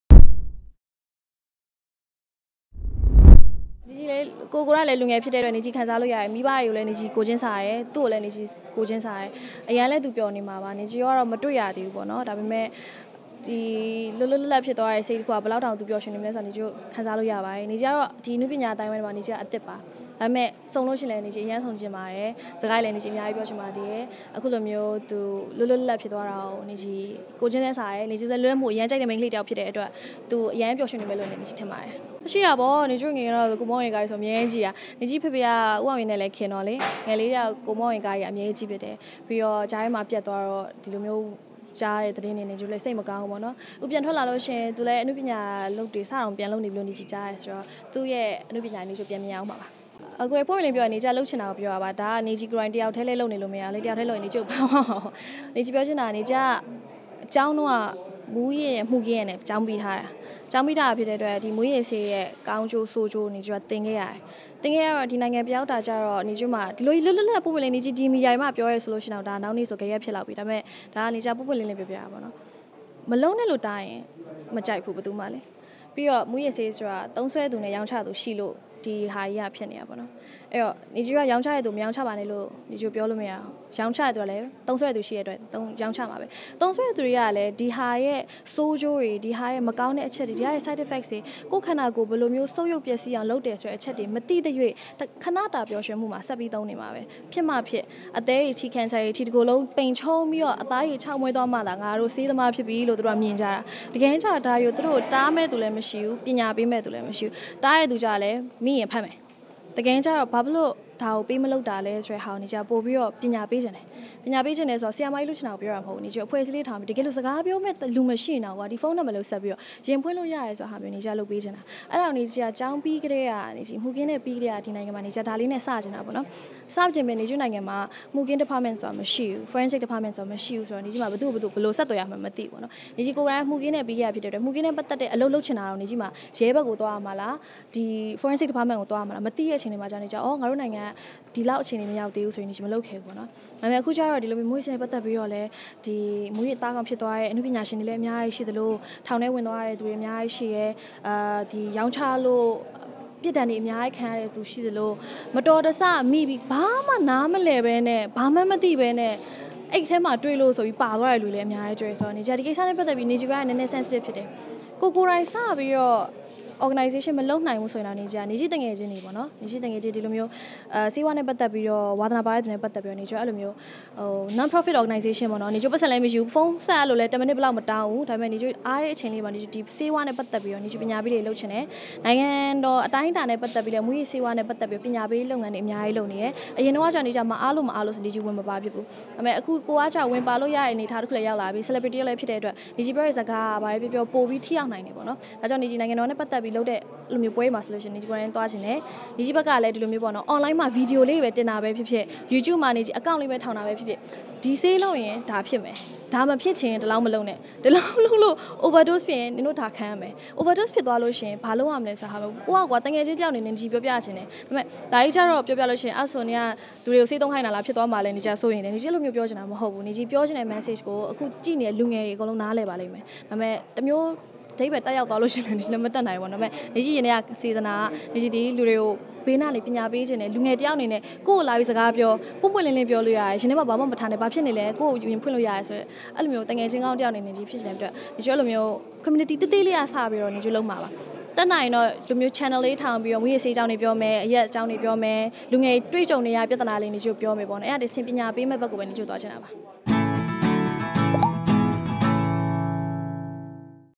မူးယစ္ေဆးဝါးနဲ႔ ပတ္သက္ၿပီး ပညာေပးအစီအစဥ္ေတြ ျပဳလုပ္ခ်င္တဲ့အေၾကာင္း ေျပာျပလာတဲ့ ေနျခည္ဦး။
"ေနျခည္ အဲလိုမ်ိဳး Community ေသးေသးေလးကစၿပီး ေနျခည္ လုပ္မွာပါ၊ တတ္ႏိုင္ရင္ေတာ့ ဒီလိုမ်ိဳး Channel ေလးေထာင္ၿပီးေတာ့၊ မူးယစ္ေဆးအေၾကာင္းေတြေျပာမယ္၊အရက္အေၾကာင္းေတြေျပာမယ္၊လူငယ္ေတြေတြ႔ၾကံဳေနရတဲ့ ျပႆနာေတြကို ေနျခည္တို႔ ေျပာမယ္ေပါ့။ အဲကေနတစ္ဆင့္ ပညာေပးမယ့္ ဘက္ကိုဘဲ ေနျခည္တို႔သြားခ်င္တာပါ"ဆိုၿပီး လူငယ္ေတြအတြက္ မူးယစ္ေဆးဝါးနဲ႔ ပတ္သက္ၿပီး ပညာေပးအစီအစဥ္ေတြ ျပဳလုပ္ခ်င္တဲ့အေၾကာင္း ေျပာျပလာတဲ့ ေနျခည္ဦး။